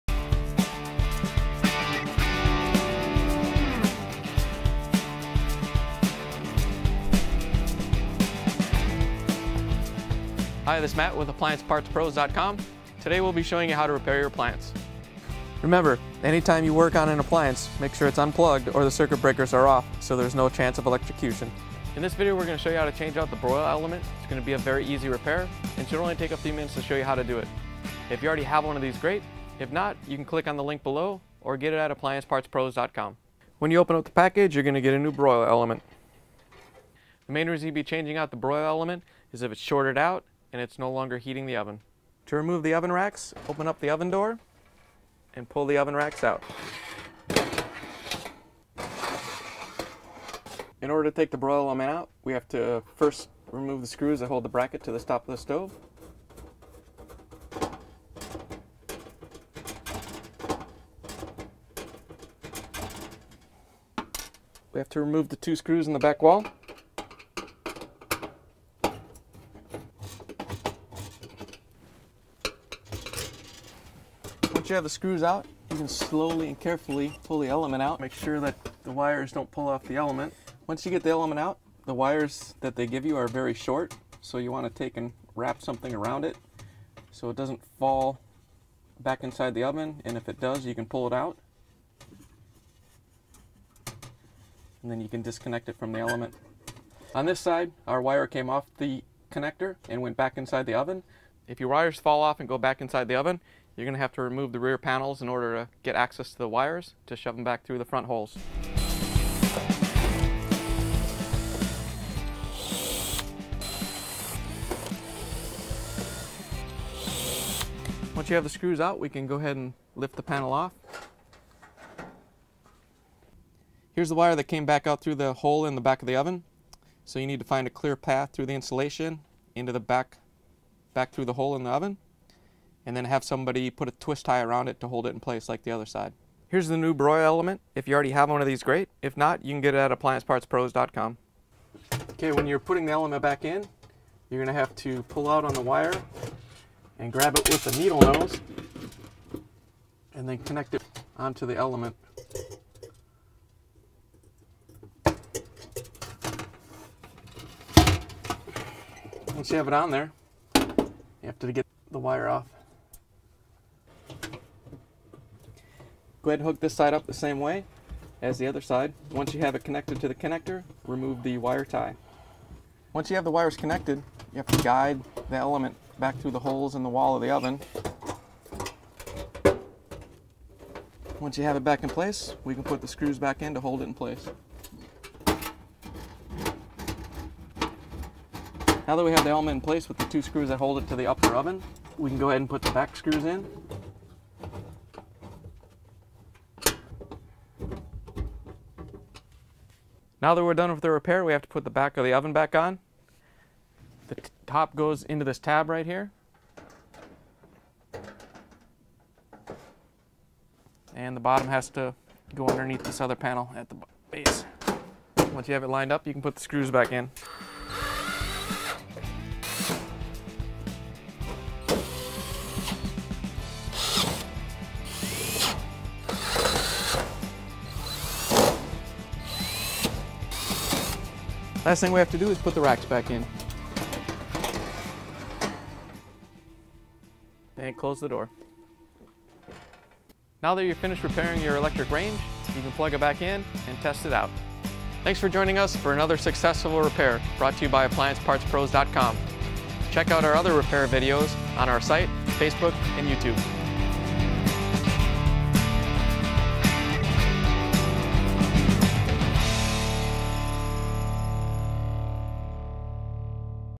How To: Oven Broil Element
Symptoms: Oven not heating on broil setting

Tools: Phillips screwdriver